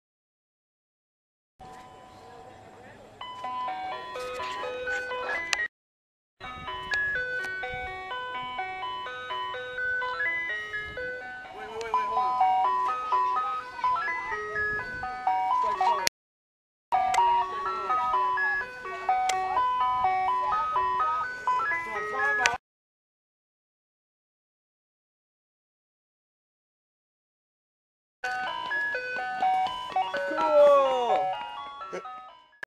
quick ice cream truck visit
Madison wisconsin  northside    Ice cream truck stops by right when we finish dinner.    yea nice  audio track